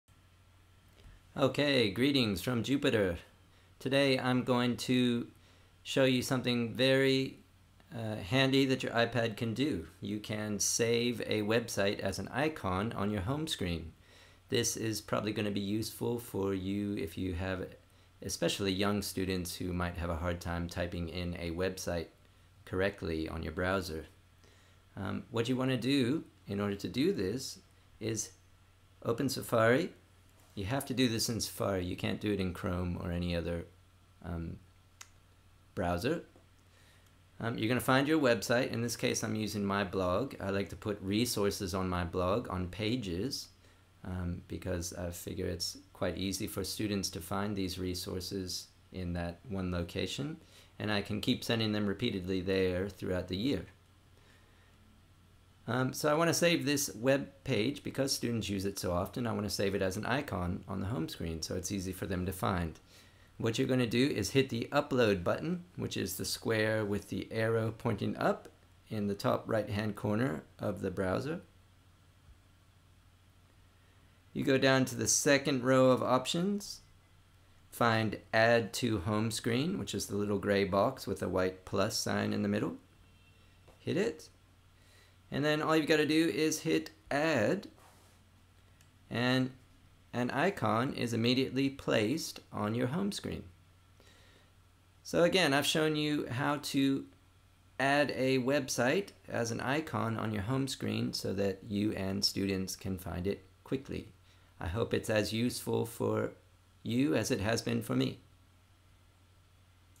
How to Add a Website as an Icon on an iPad Homescreen
This little video will show you how to add a website as an icon on your iPad. Great for younger students who might have difficulty typing in the name of a complicated website. Also great for navigating to sites that you visit often.